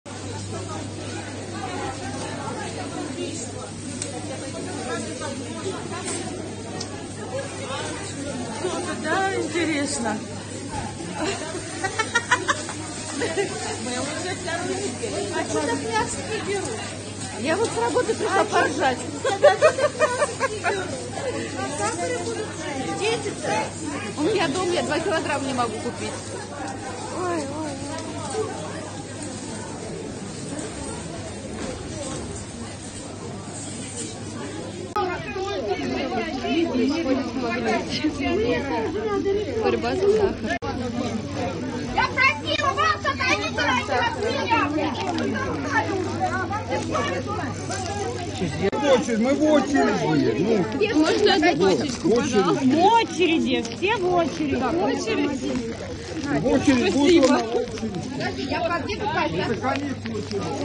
В Рязанській і Володимирській областях росії пенсіонери влаштували бійки за цукор
В Рязанской и Владимирской областях России пенсионеры устроили драки за сахар

In the Ryazan and Vladimir regions of Russia, retirees staged fights for sugar